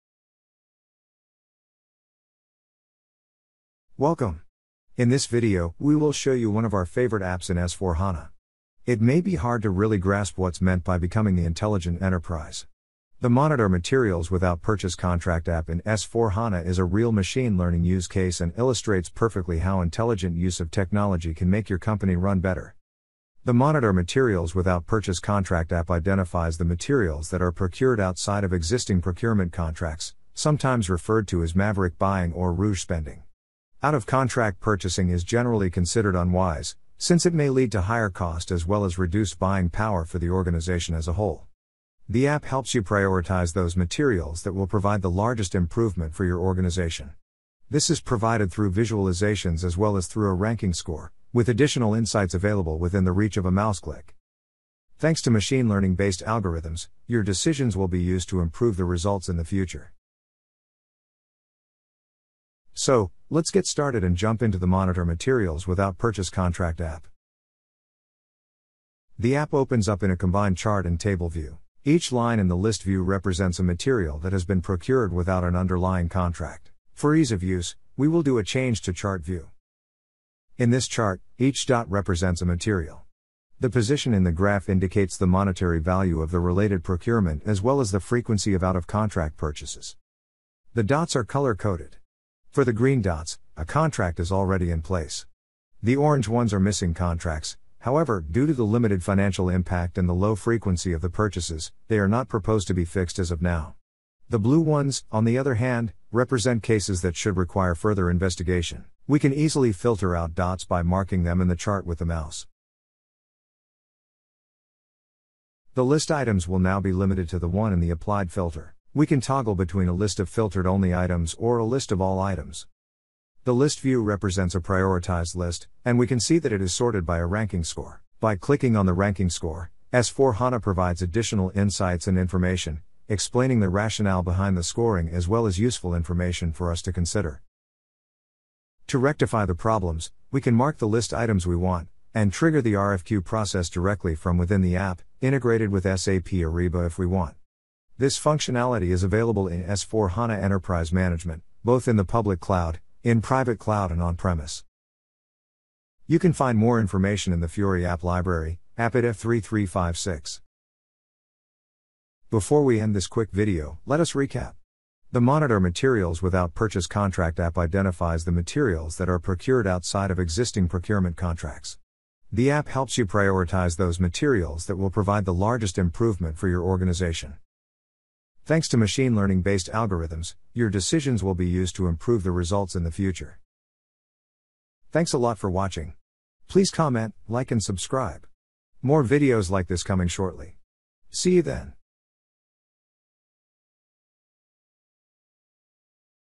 SAP S/4HANA - Monitor Materials without Purchase Contract - Favourite App!
In this video, we will show you one of our favourite apps in S4HANA. It may be hard to really grasp what's meant by "becoming the Intelligent Enterprise". The Monitor Materials without Purchase Contract app in S4HANA is a  real machine learning use case and illustrates perfectly how intelligent use of technology can make your company run better.

0:00 Introduction
0:23 Benefits
1:02 Demo
2:27 Available in
2:43 Recap Benefits